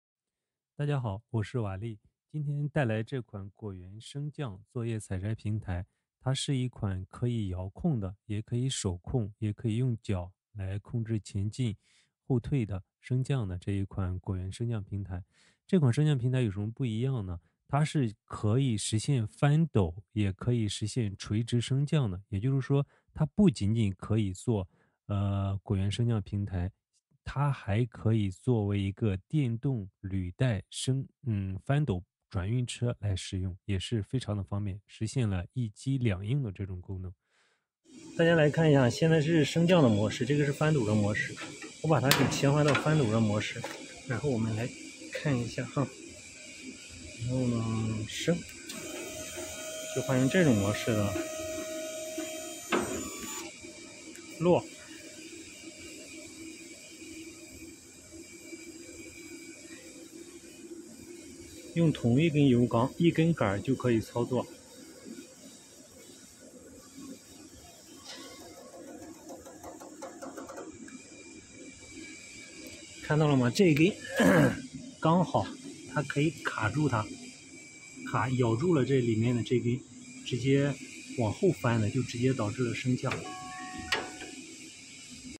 电动履带果园升降翻斗车可以升降也可以翻斗
驱动方式：双电机驱动
载重： 350千克
电池：4块铅酸电池
举升高度：1200 mm
翻斗方式：电动液压翻斗 + 垂直升降
操作方式：手控、脚控、遥控
选配功能：电动剪刀，电动打药机，电子秤